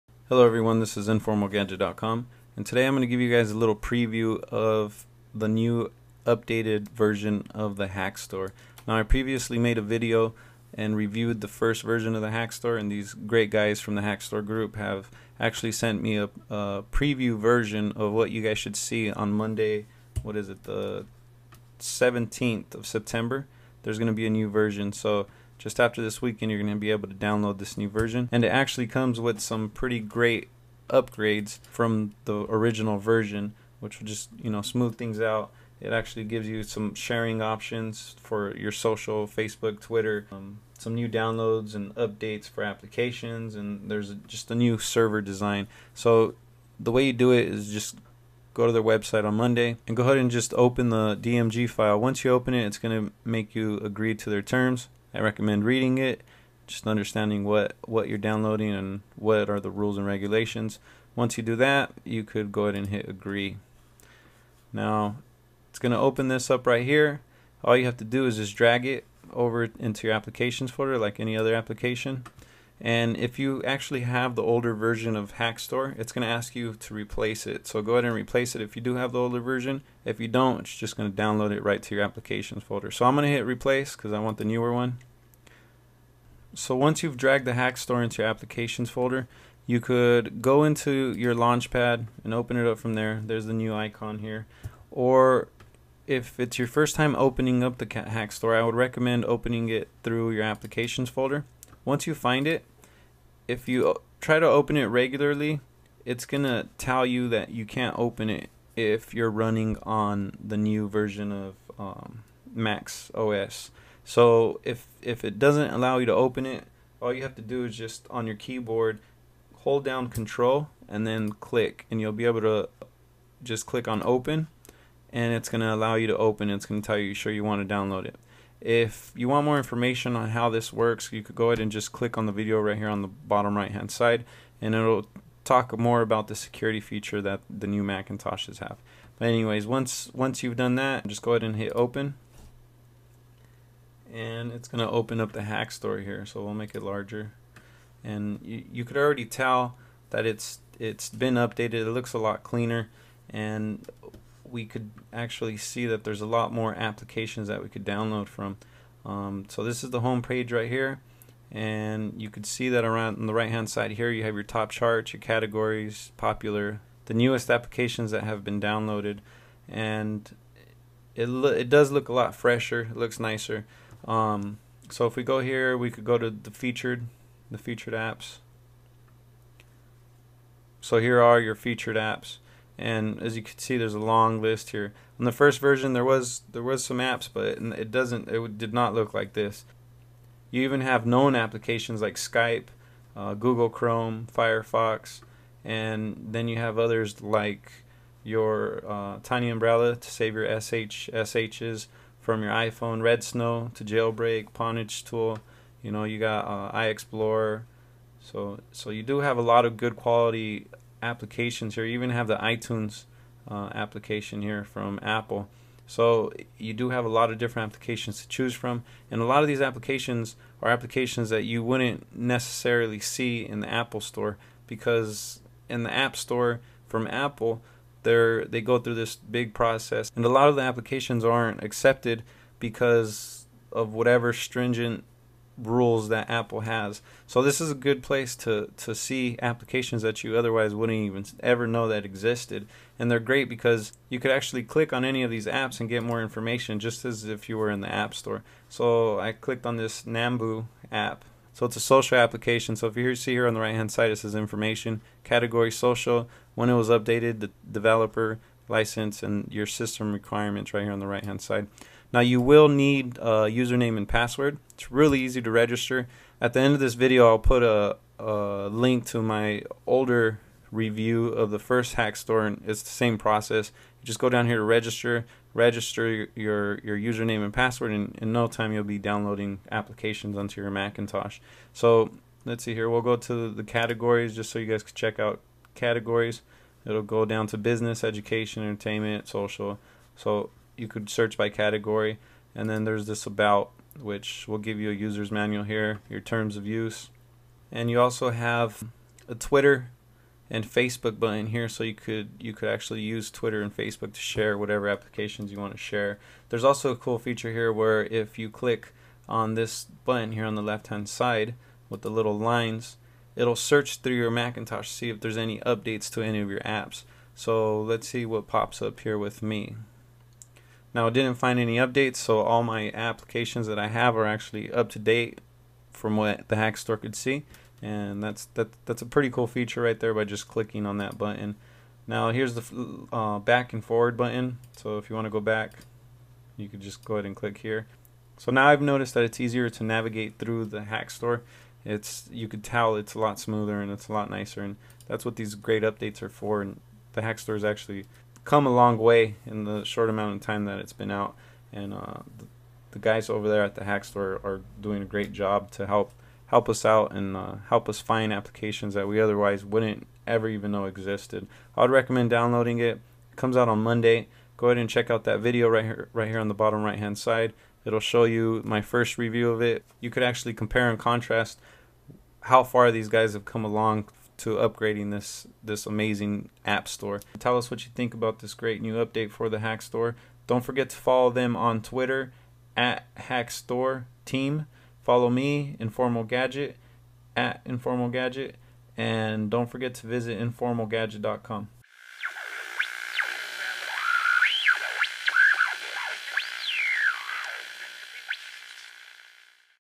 NEW HackStore AppStore For Apple Mac's, Reviewing Updated Version and How-To Install The Application
HackStore for Mac update is out! Download apps from the HackStore. The Good guys over at the Hack Store Team has allowed me to get an early sneak preview, to test it out :). Learn how to download the .DMG file and set up the Hack Store application for your Mac. Come check out the improvements!